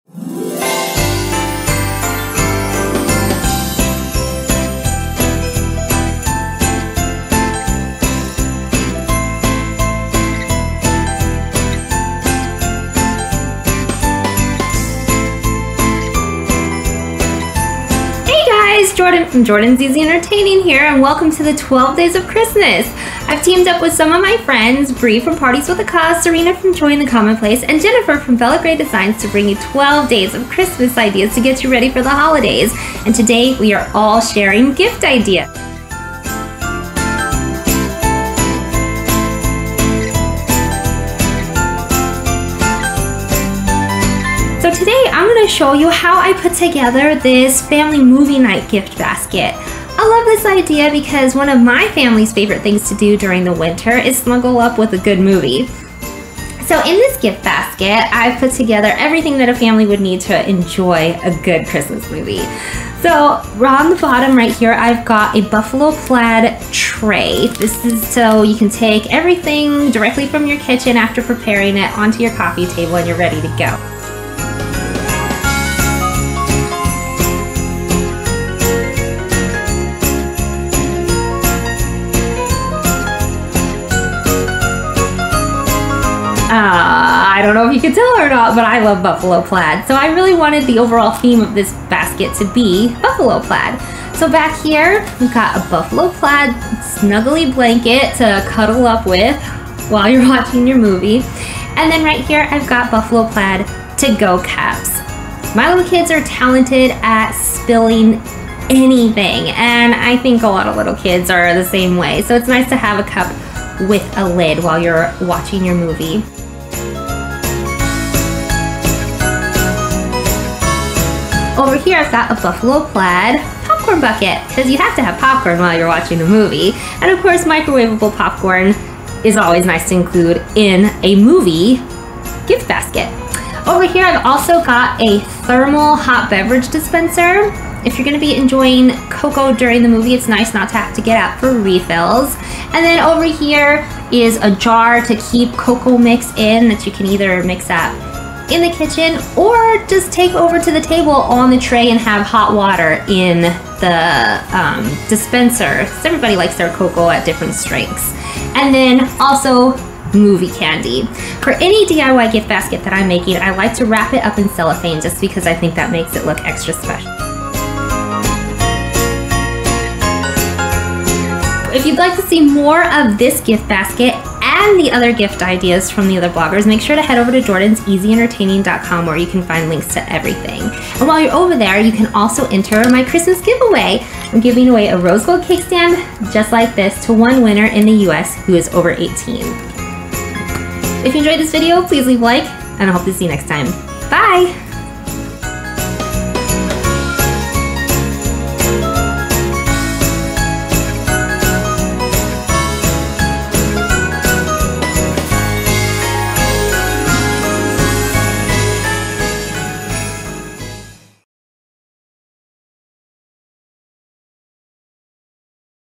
Ultimate Christmas Family Movie Night DIY Gift Basket!
Welcome to day 6 of the 12 Days of Christmas!  Today the other bloggers and I are sharing gift ideas.  I'm showing you everything that I put into this family movie night diy gift basket.  I love this idea for families who enjoy watching Christmas movies together.  It's also a great gift idea for a boyfriend, neighbor, or any movie lover!

See more of this family movie night gift basket, get links to the other blogger's gift ideas, and enter my Christmas giveaway at: